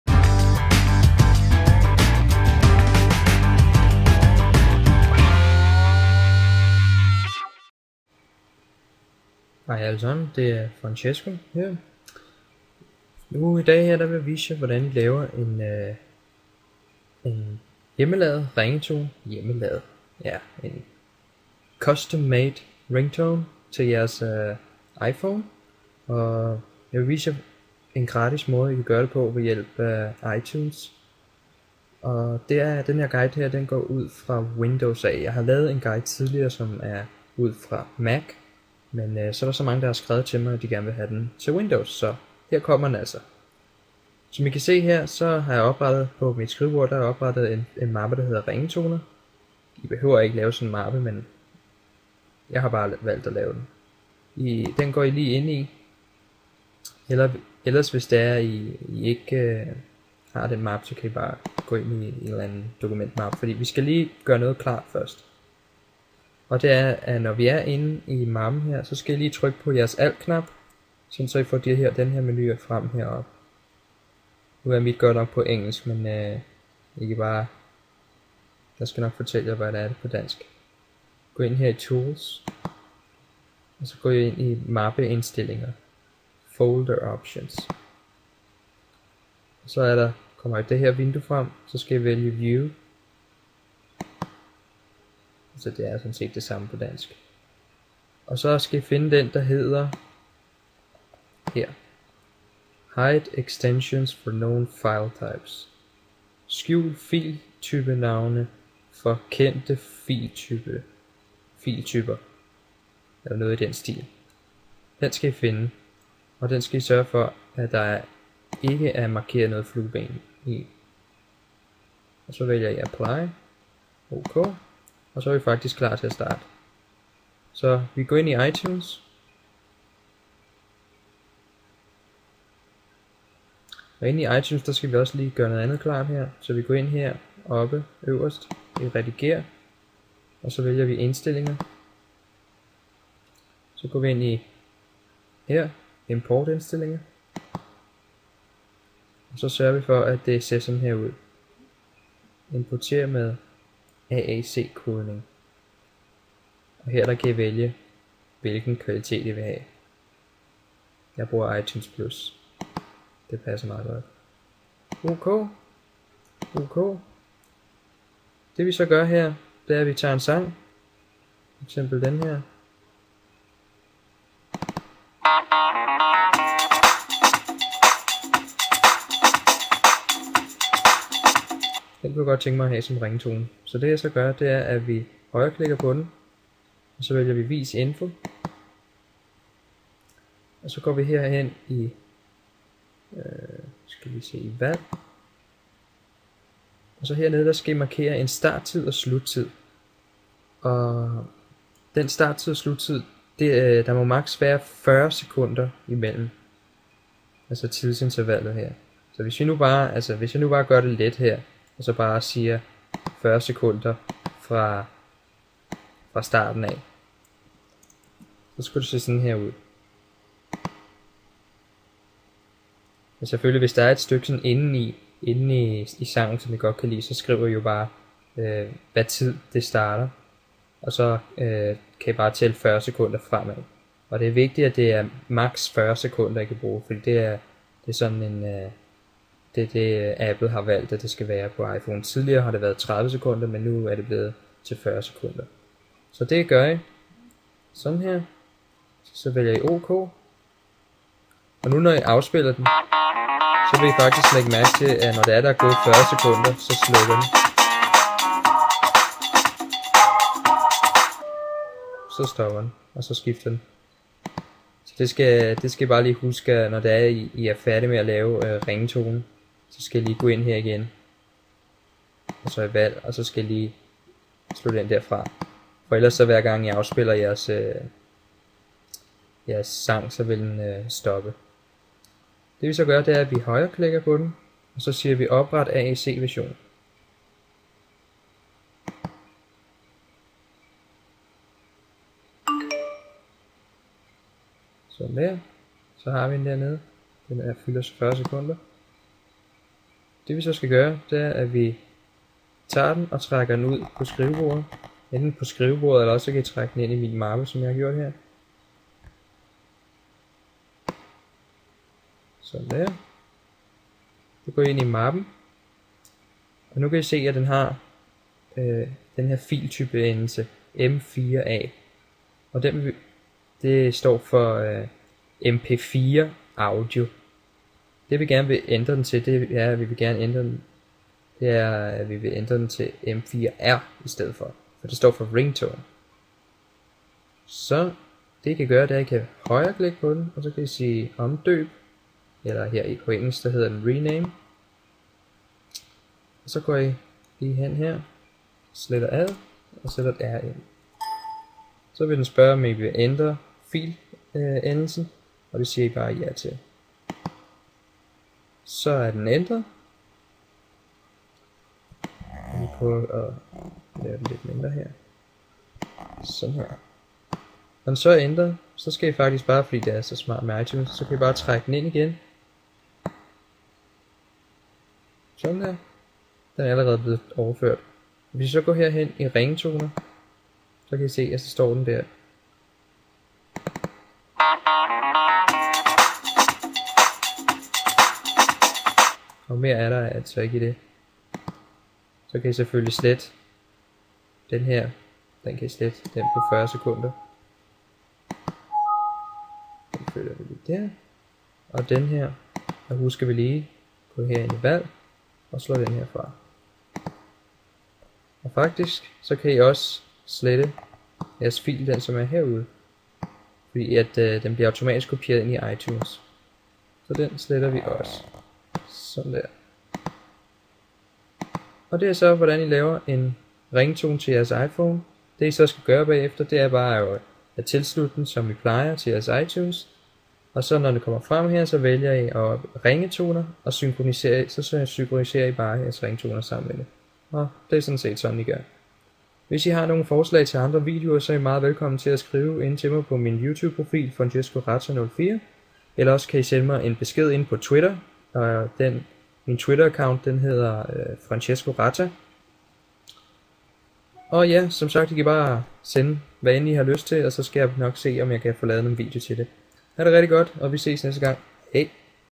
Opret dine egne ringetoner til din iPhone gratis med iTunes
I denne video, vil jeg forklare hvordan i kan lave hvilken som helst af jeres sange om til en ringetone som i kan overføre over til jeres iPhone.

Guiden er blevet lavet til Windows da jeg har fået en del anmodninger fra flere brugere der gerne ville have guiden i Windows samt i HD, så her er den så! :)